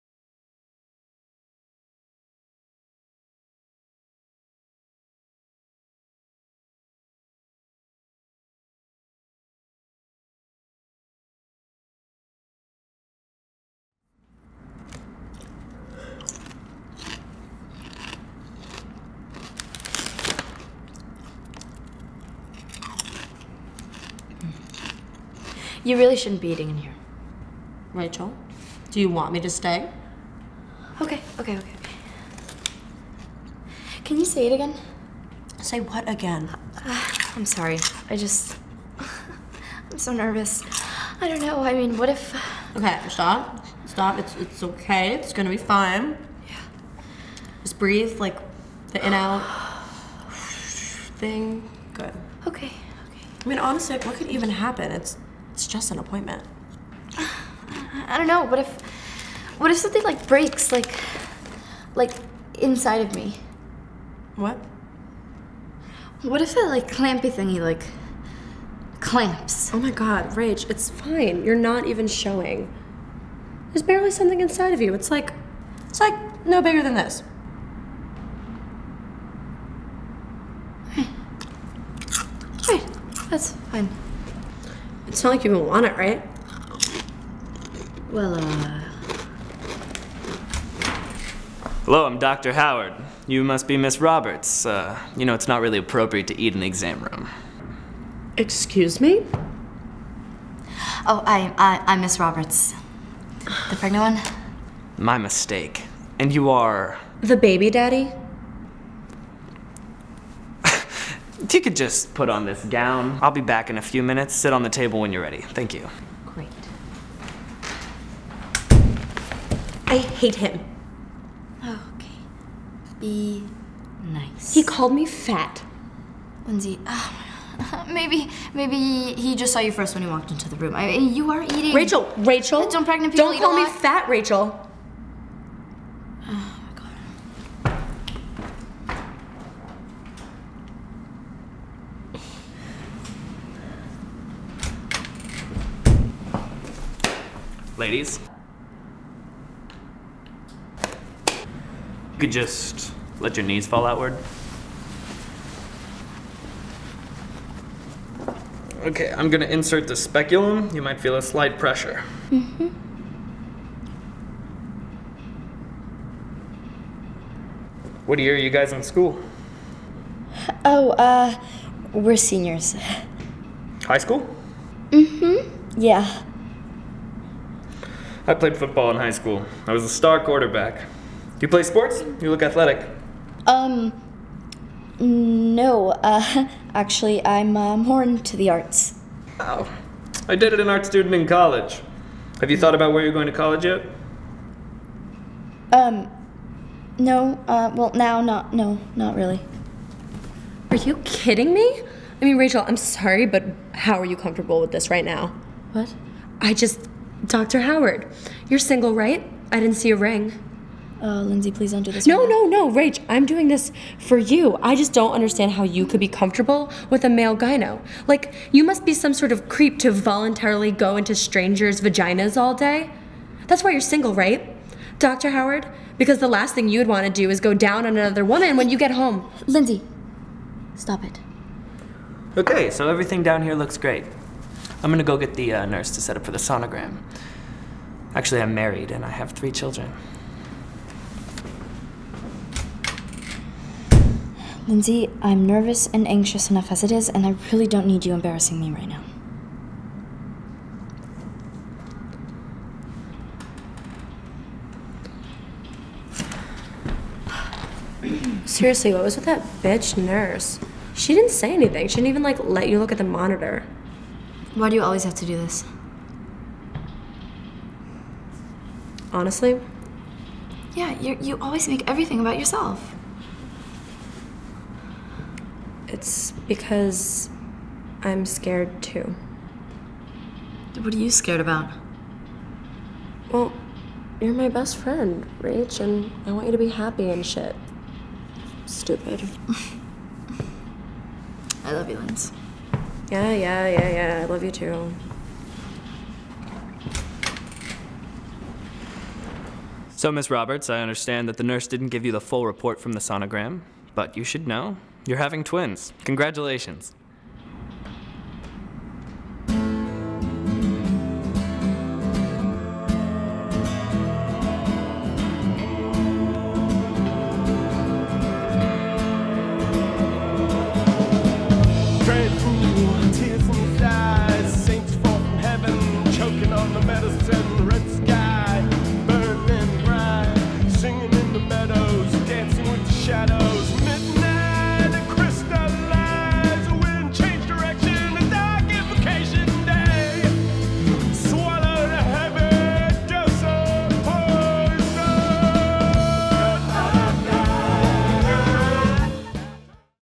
Playing Hookie
A short I wrote about two high school best friends trying to figure out morality. Produced by Stonestreet. Starring me, Megan Tomei, Alexa Halvorssen, and Jared Svoboda. Enjoy!